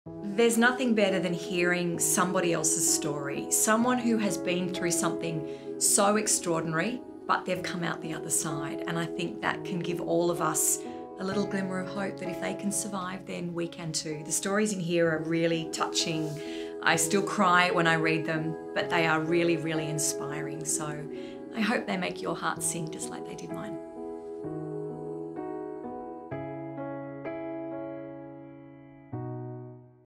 Fifteen Seconds of Brave by Melissa Doyle
In your darkest hour, can you still find hope?

In this intimate and insightful book, Melissa Doyle shares the stories of some of the most resilient people she has ever met, gently drawing out their wisdom, empathy and heartfelt practical advice for anyone who’s going through a difficult time.